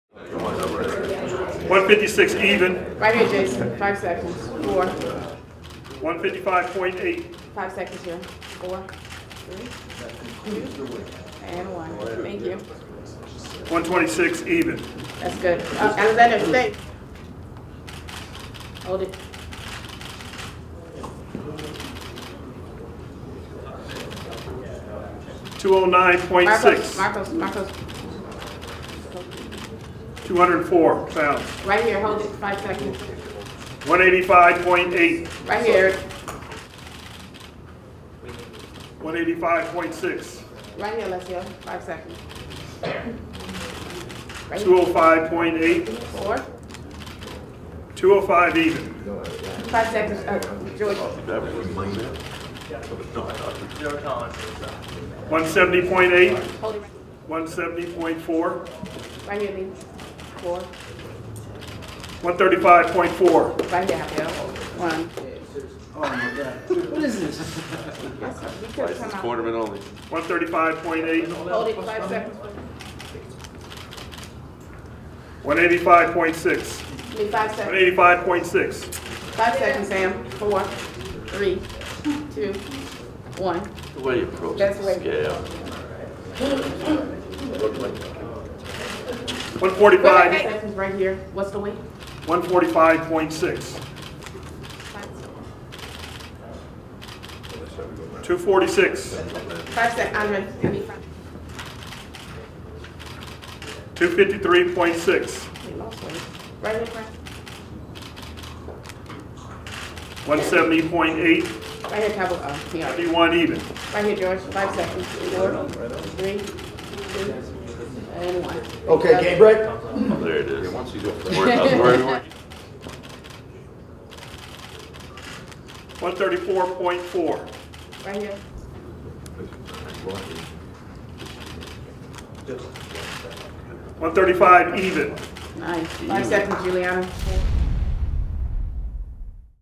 Valentina Shevchenko and Julianna Pena official for UFC on Fox 23 scrap
All 24 fighters but one made weight for their scheduled bouts in Denver. See their official weigh-ins on the scale.